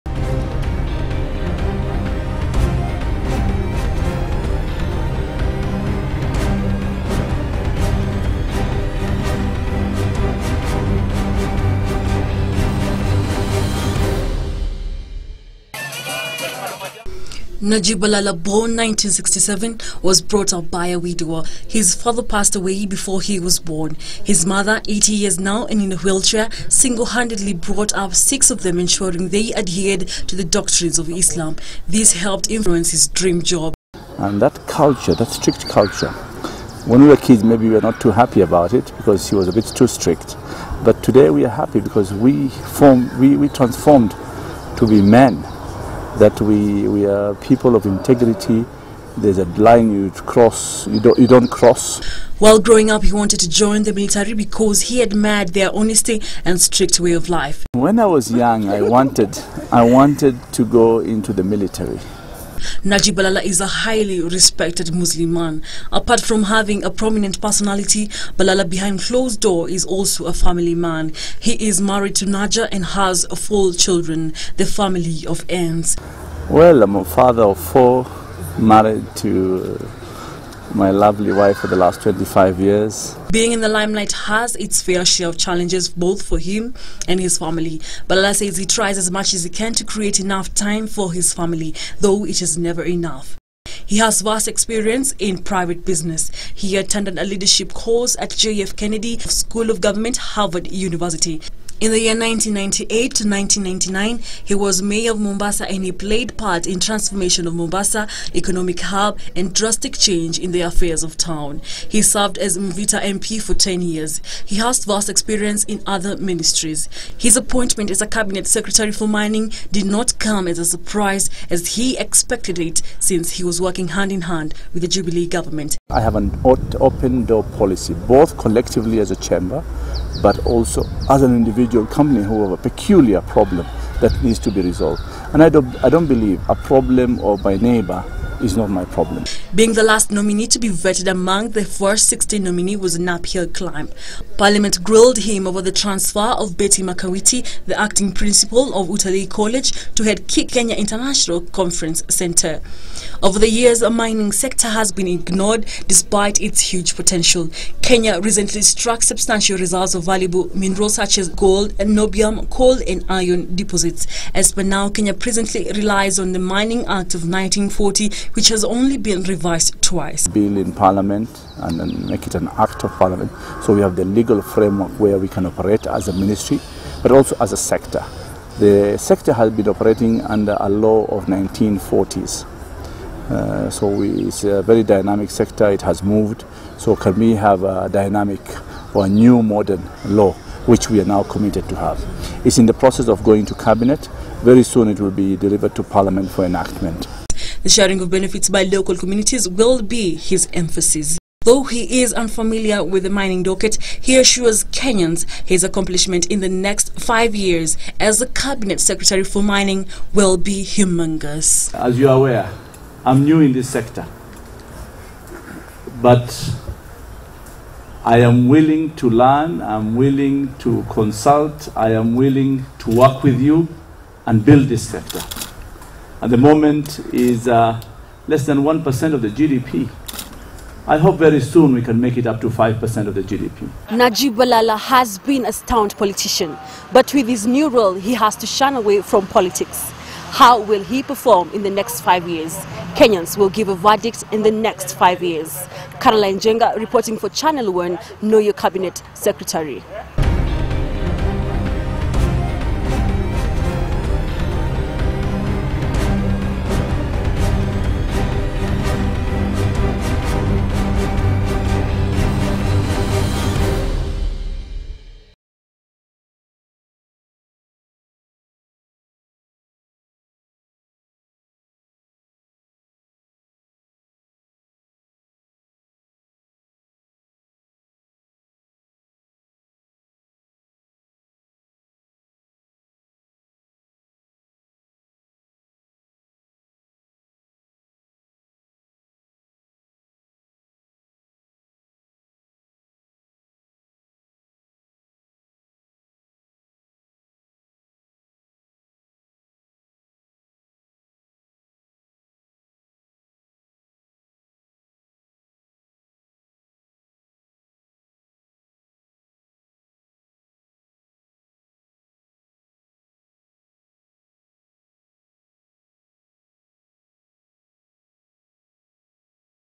KNOW YOUR CABINET SECRETARY-Najib Balala
While growing up Najib Balala wanted to join the military because he admired their honest and strict way of life. But as fate would have it, he found himself in politics starting off as Mombasa counselor who was to be elected mayor of the second largest town in Kenya. And as they say the rest of his political career is history. He is now Kenya's mining cabinet secretary. Carol Njenga brings us more details.